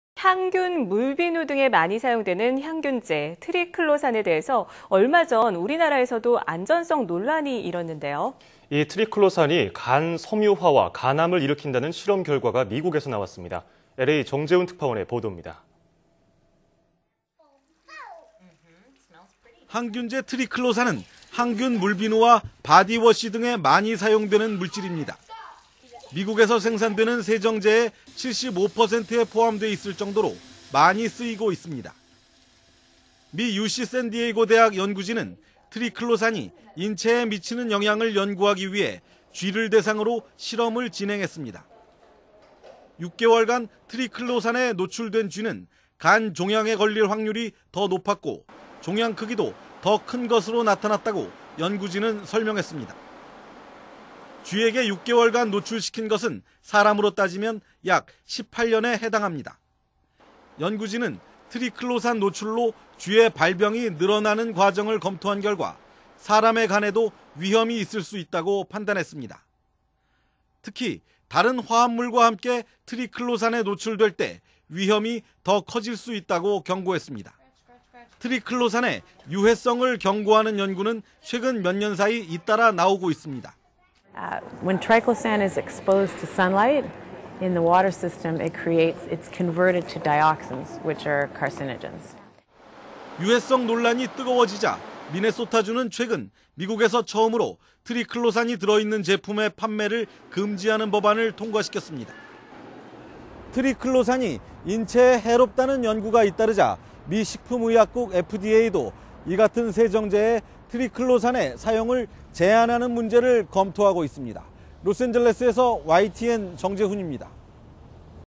"항균 물비누 '트리클로산' 간암 등 유발" / YTN 사이언스
[앵커] 항균 물비누 등에 많이 사용되는 항균제 '트리클로산'이라는 물질, 얼마전 우리나라에서도 안전 논란이 일었는데요,'트리클로산'이 간섬유화와 간암을 일으킨다는 실험 결과가 미국에서 나왔습니다.LA 정재훈 특파원이 보도합니다.[기자]항균제 '트리클로산'은 항균 물비누와 바디워쉬 등에 많이 사용되는 물질입니...